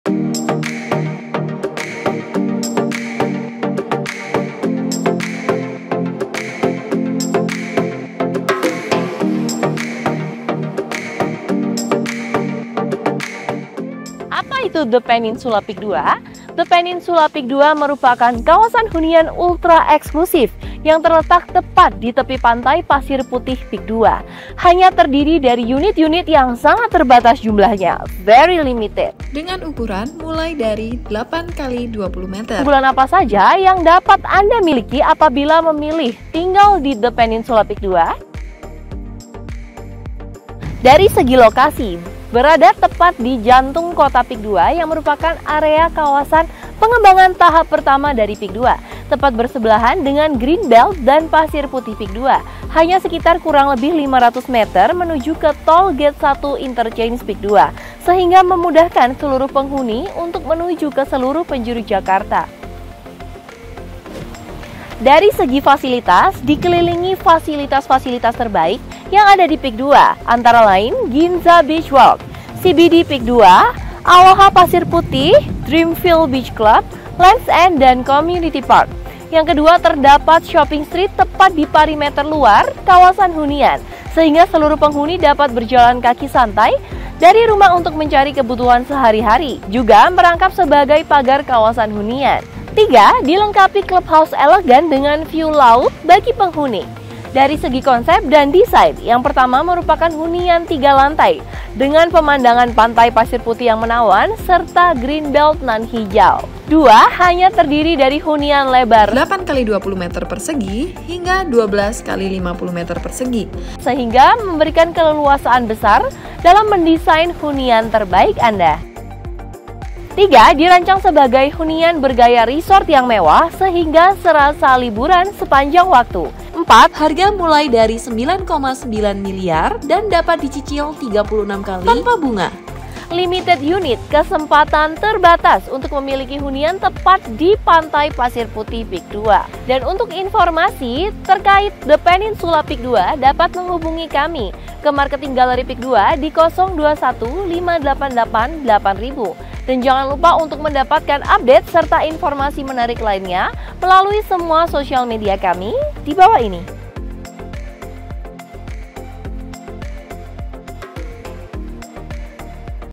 Kawasan Hunian Eksklusif Di Tepi Pantai Pasir Putih PIK2!
The Peninsula PIK2, kawasan hunian eksklusif yang berlokasi di tepi Pantai Pasir Putih PIK2 akan segera hadir! Nantinya kawasan ini akan memiliki lokasi yang sangat strategis sehingga memudahkan akses untuk bepergian kemanapun.

The Peninsula PIK2 juga akan memilik konsep dan desain yang unik, sehingga Anda bisa merasakan liburan sepanjang waktu!